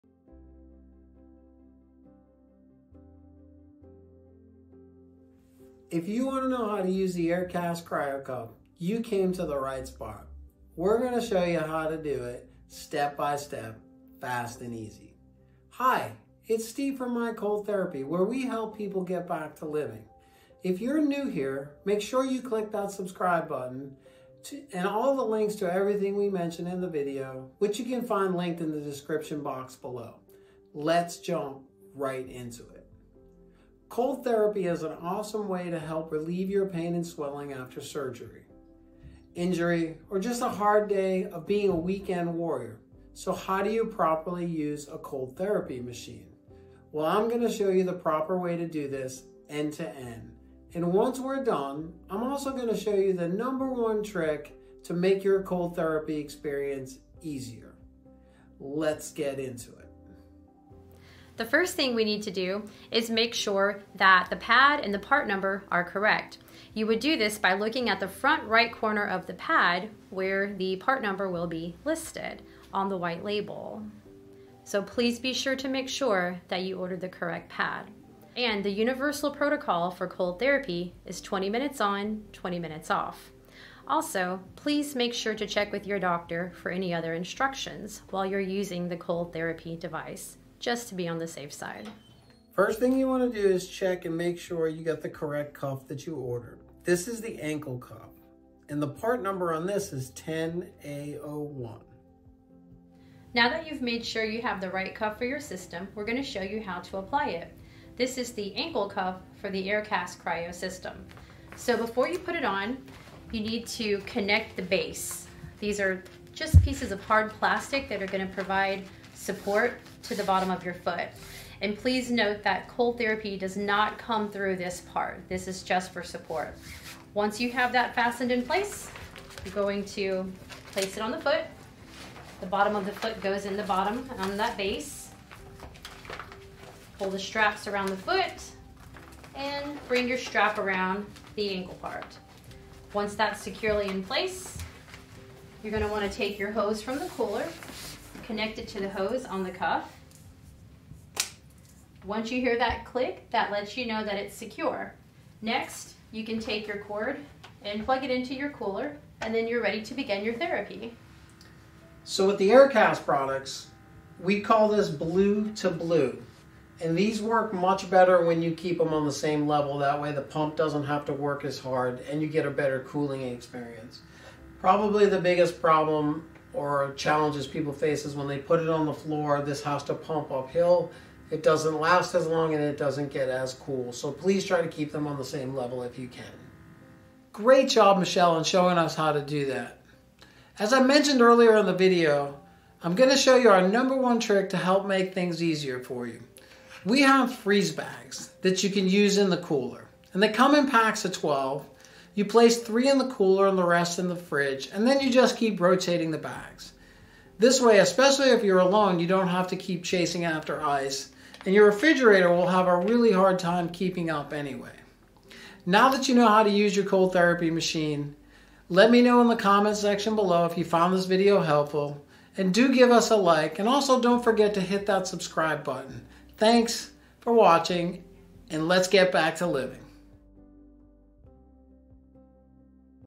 Expert Tips: Aircast Cryo Ankle Cuff Tutorial
How to Apply the Aircast Cryo Ankle Cuff

In this video, we will show you step-by-step instructions on how to properly apply the Aircast Cryo Ankle Cuff for maximum effectiveness and comfort. The Aircast Cryo Ankle Cuff is designed to provide targeted cold therapy to help reduce pain and swelling in the ankle area. Whether you are recovering from an injury or simply looking for relief from everyday aches and pains, the Aircast Cryo ankle cuff can be a great tool to have in your recovery arsenal. Follow along as we demonstrate the proper way to apply the cuff, including securing it in place. With the help of this video, you'll be able to make the most out of your Aircast Cryo Ankle Cuff and experience the benefits of cold therapy firsthand. Don't let ankle pain hold you back any longer - watch our tutorial and learn how to apply the Aircast Cryo Ankle Cuff today!

Timestamps:
0:00 What is Cold Therapy?
1:00 Helpful Tips
1:32 Aircast Cryo Ankle Cuff Part Number
1:42 How to Apply the Aircast Cryo Ankle Cuff
3:07 Using Freeze Bags
3:35 Closing Remarks